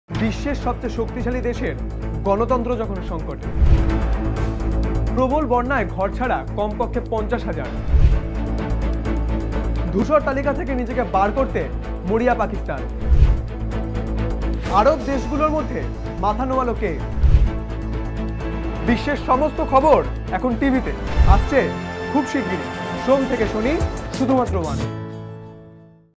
Viswa Bulletin | New Program
It's a NEWS BULLETIN program, with world news information. It will be on aired everyday.